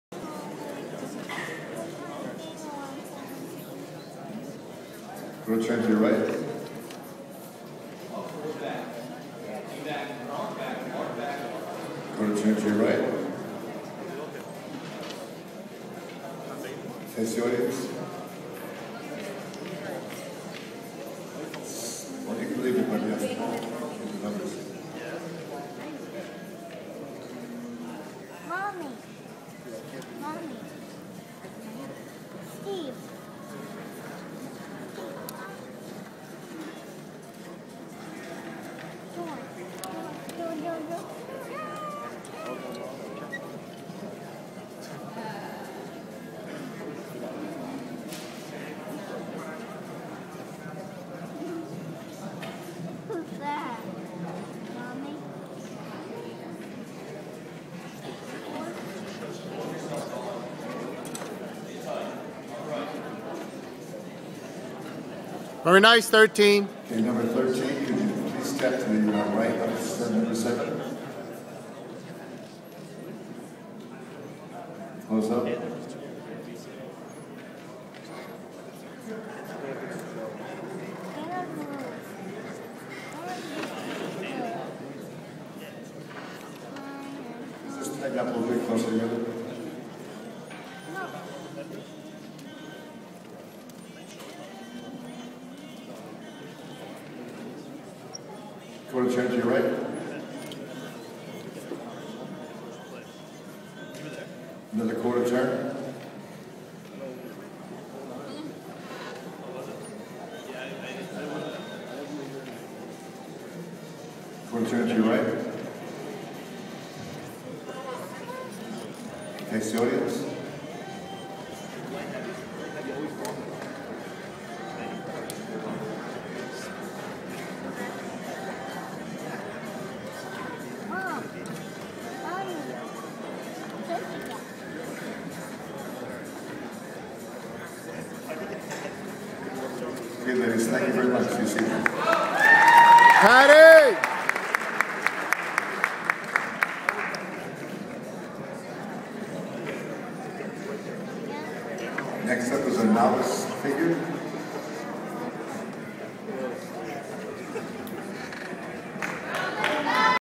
Patricia Jerry DiGiovanni women's masters of Gold's Classic 2011 in Brick Memorial High School, Brick, NJ.